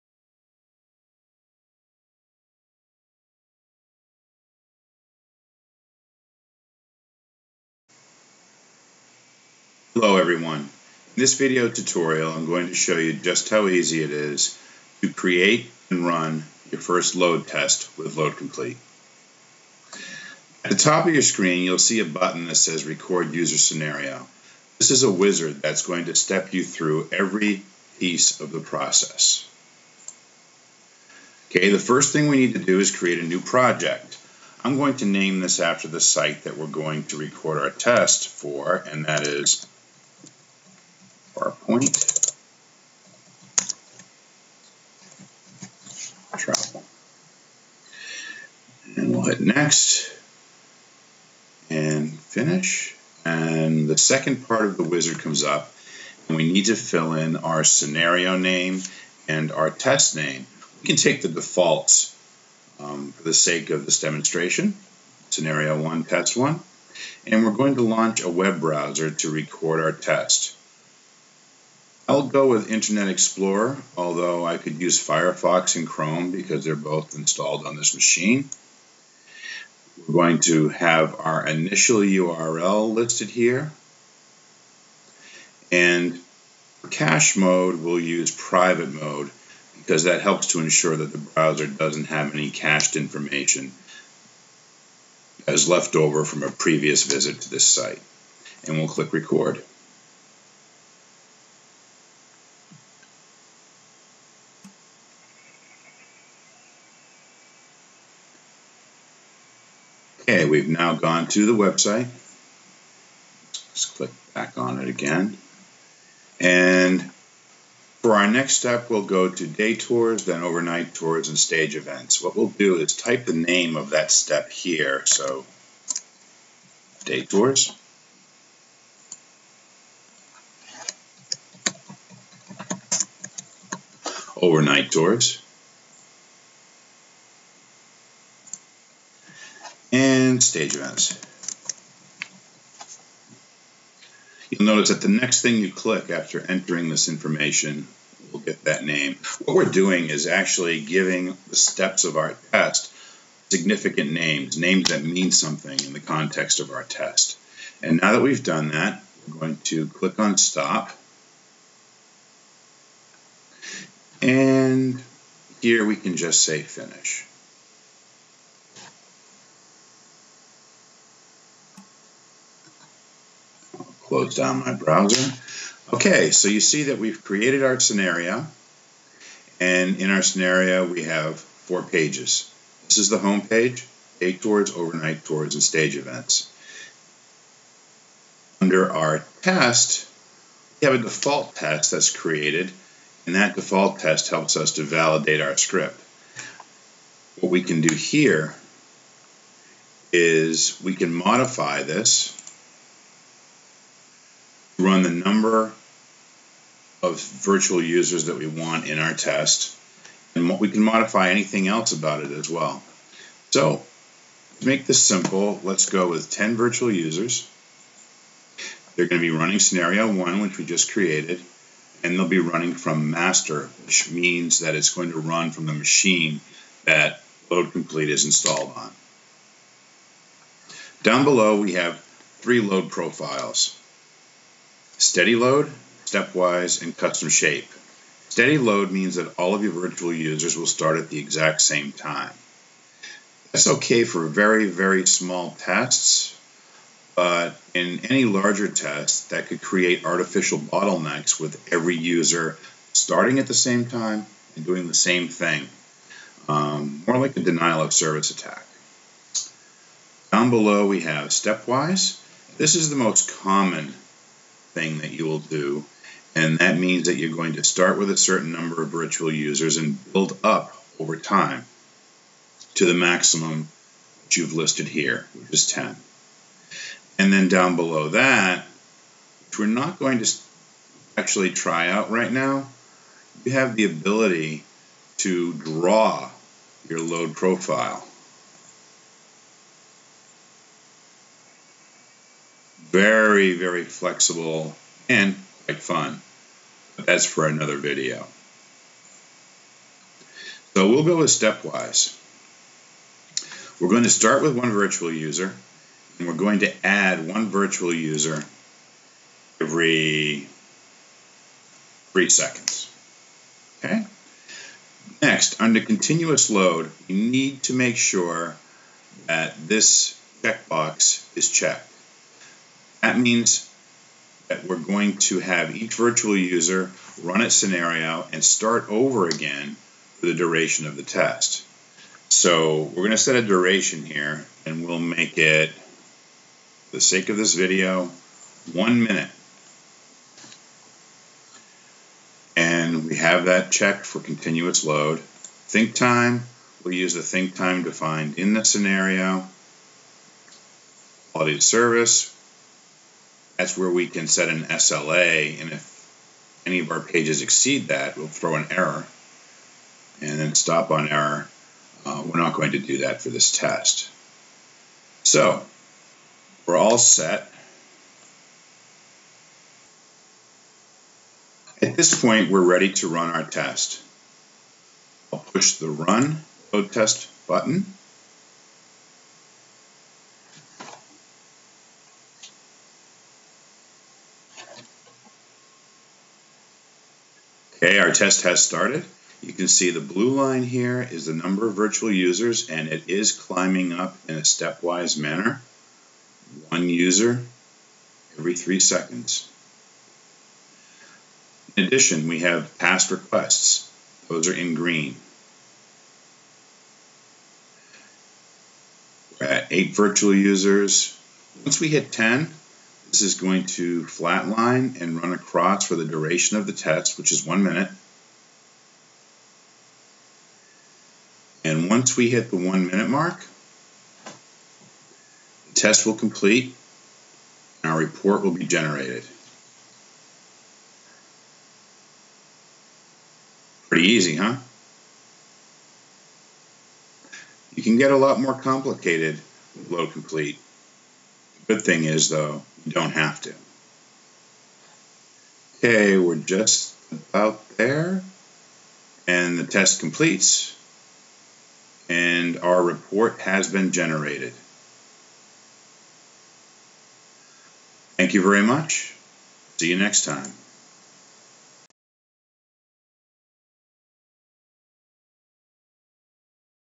How to Create Your First Test in LoadComplete
Get started in LoadComplete by creating your first load test with this quick tutorial.

LoadComplete is a desktop tool for load, stress, scalability testing of websites and web applications. Designed from the ground-up to help you focus on the tasks at hand, it does not require advanced programming skills. It works with both traditional HTML web sites and with Rich Internet Applications using modern web technologies like AJAX, ASP.NET, Java, Flash, Flex and Silverlight. The 'record and playpack' and 'visual programming' features enable you to create powerful and flexible first load test within a few seconds.